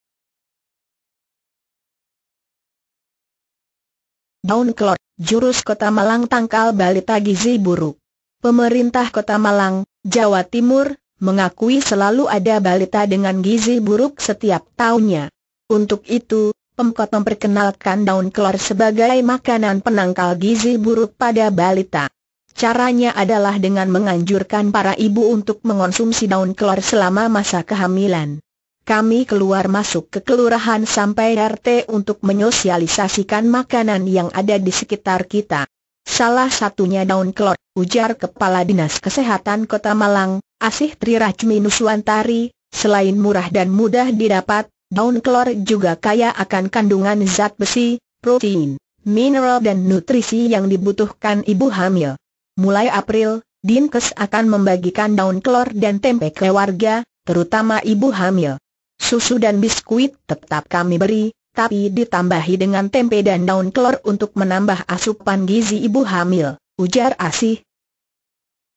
Khasit Daun Kelor Bagi Ibu Hamil yang Sangat Besar
Daun Kelor, Jurus Kota Malang Tangkal Balita Gizi Buruk

Pemerintah Kota Malang, Jawa Timur, mengakui selalu ada balita dengan gizi buruk setiap tahunnya. Untuk itu, pemkot memperkenalkan daun kelor sebagai makanan penangkal gizi buruk pada balita.

Caranya adalah dengan menganjurkan para ibu untuk mengonsumsi daun kelor selama masa kehamilan.

"Kami keluar masuk ke kelurahan sampai RT untuk menyosialisasikan makanan yang ada di sekitar kita. Salah satunya daun kelor," ujar Kepala Dinas Kesehatan Kota Malang, Asih Trirachmi Nuswantari, Selain murah dan mudah didapat, daun kelor juga kaya akan kandungan zat besi, protein, mineral dan nutrisi yang dibutuhkan ibu hamil. Mulai April, Dinkes akan membagikan daun kelor dan tempe ke warga, terutama ibu hamil.

"Susu dan biskuit tetap kami beri, tapi ditambahi dengan tempe dan daun kelor untuk menambah asupan gizi ibu hamil," ujar Asih.